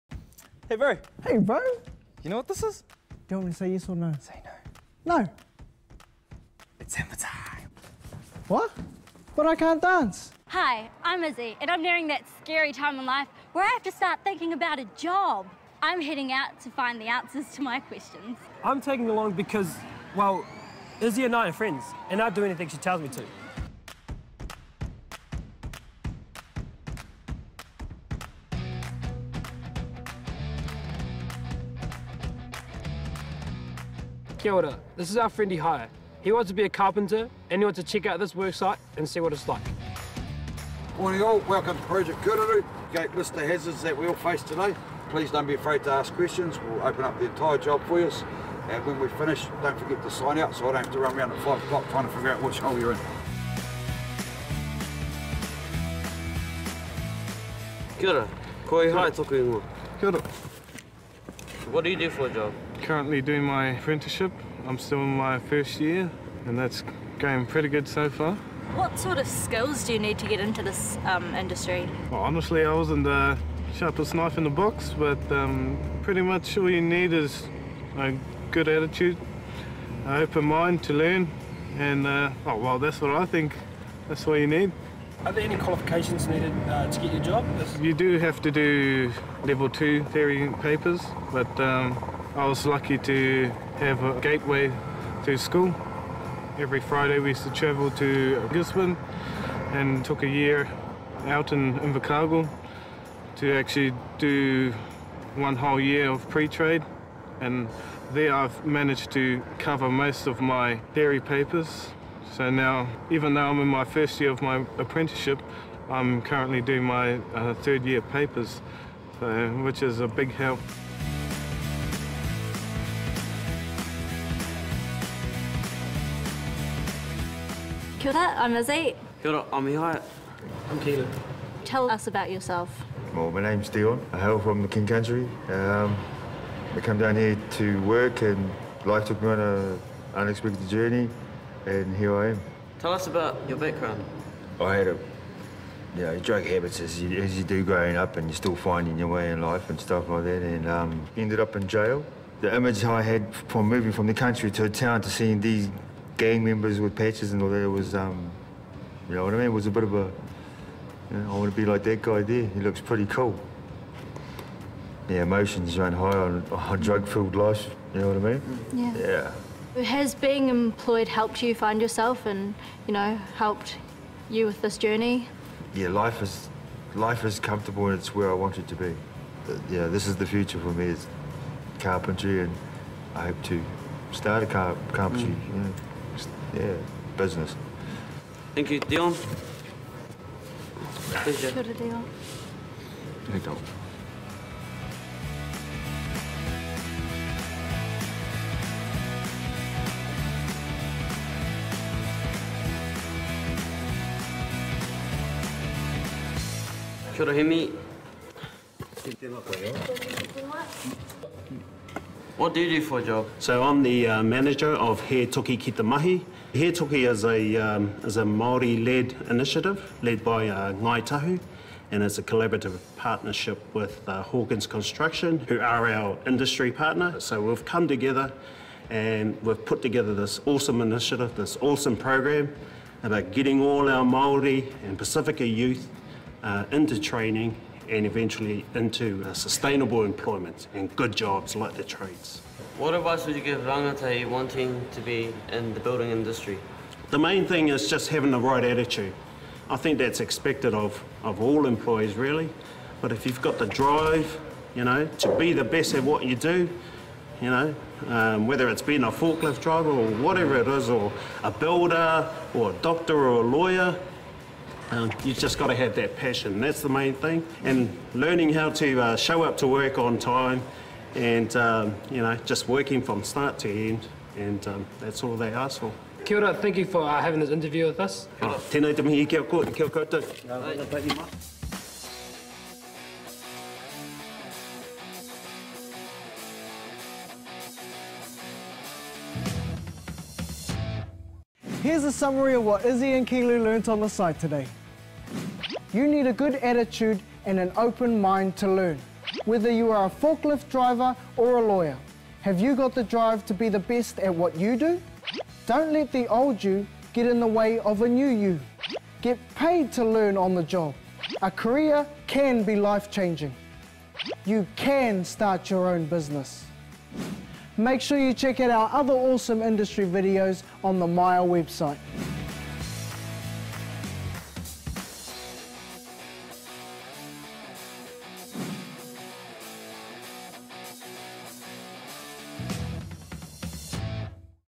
Are you keen to earn while you learn?
In this video we follow Dion, a carpentry apprentice. Dion had a tough journey, but now sees a bright future in carpentry, earning along the way and gaining mad skills.